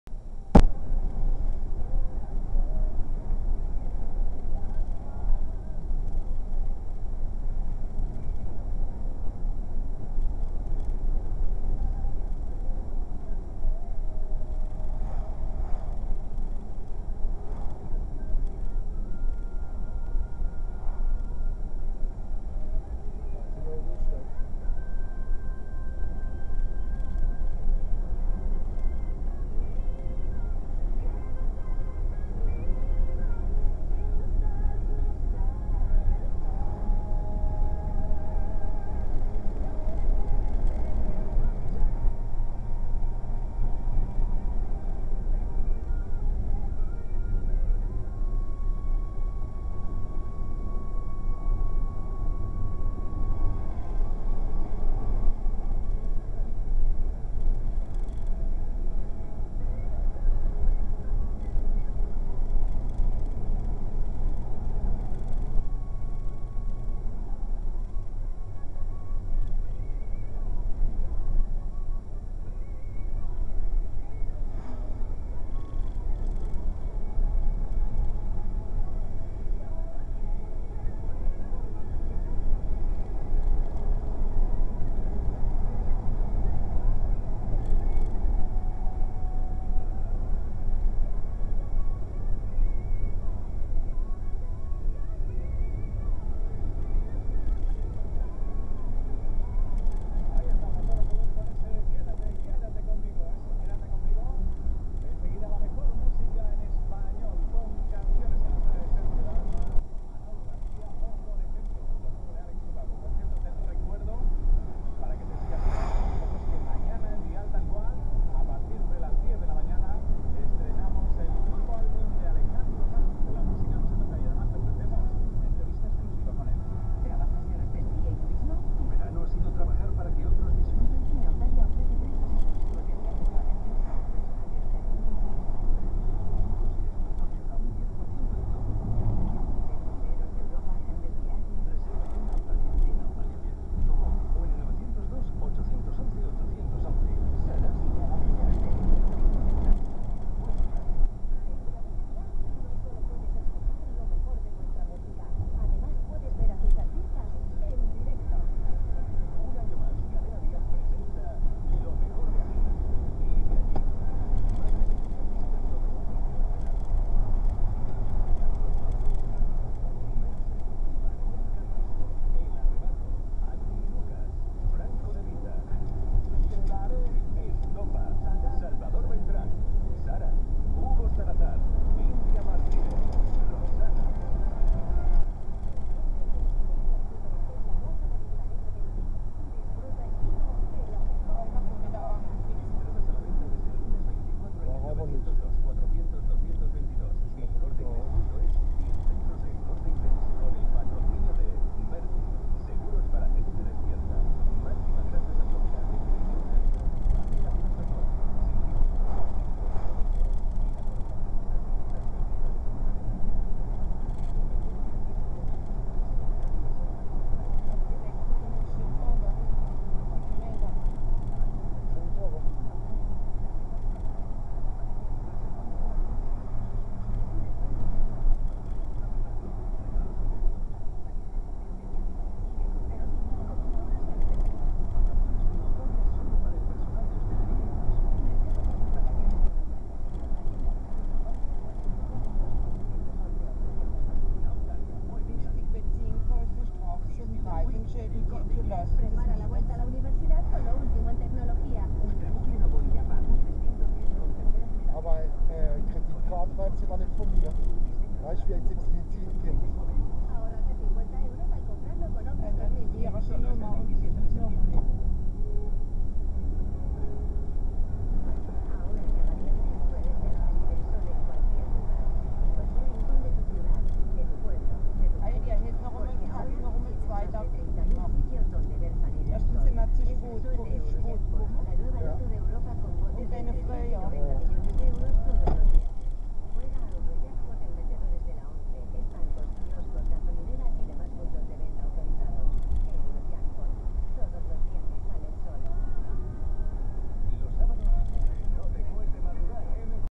Perle2012B
Gemütliche Fahrt von Amagar richtung Tazacorte.